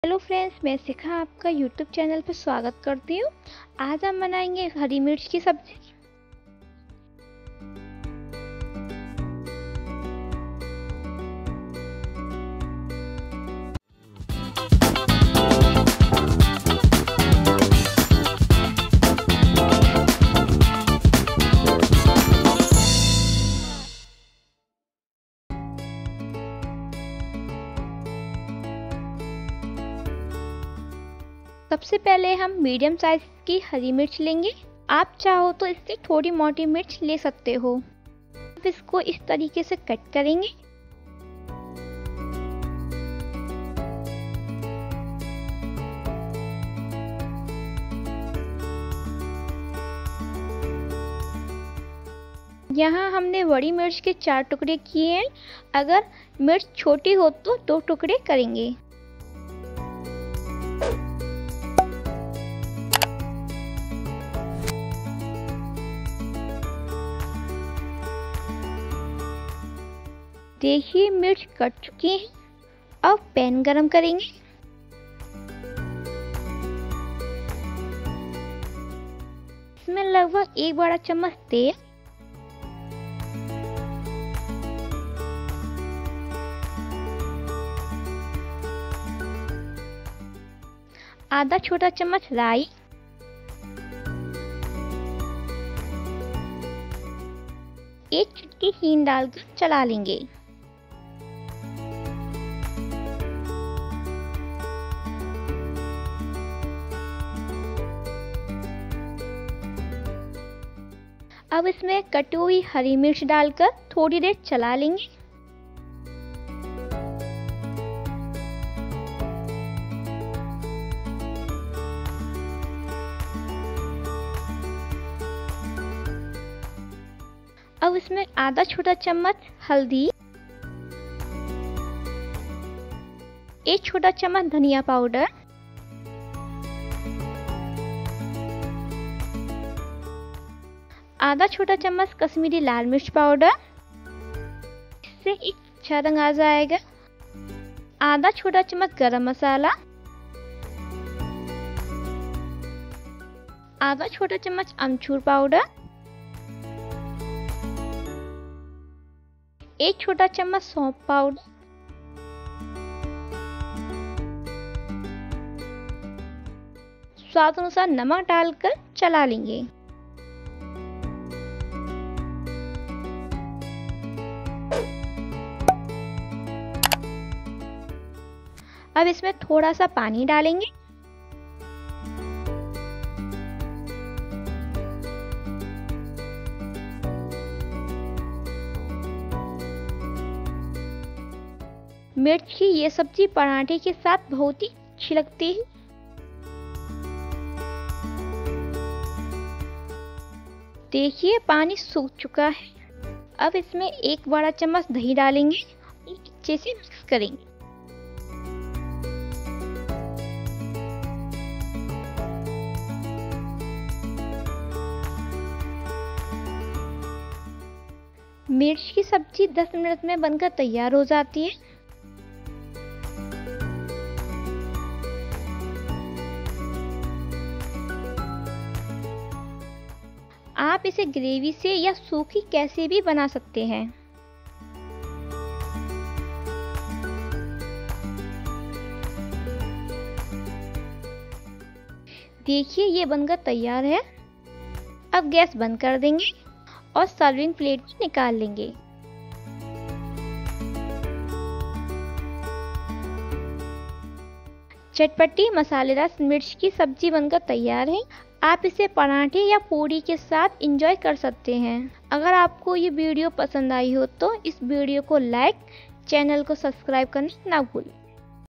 #158 Spicy Mirch ki Sabji | चटपटी मसालेदार मिर्च की सब्जी | Green Chilli Recipe
Ingredients:

हरी मिर्च (Green Chilli): 500gram
तेल (Oil) : 1tbsp
राई (Mustard Seeds): 1/2tsp
हींग (Asafoetida): 1pinch
हल्दी पाउडर (Turmeric Powder) : 1/2tsp
धनिया पाउडर (Coriander Powder) : 1tsp
कश्मीरी लाल मिर्च पाउडर (Kashmiri Red Chilli Powder) : 1/4tsp
गरम मसाला (Garam Masala) : 1/2tsp
अमचूर पाउडर (Mango Powder) : 1/2 tsp
सौंफ पाउडर (Fennel Powder) : 1tsp
नमक (Salt) : 1tsp
पानी (Water) : 2 tbsp
दही (Curd) : 1tbsp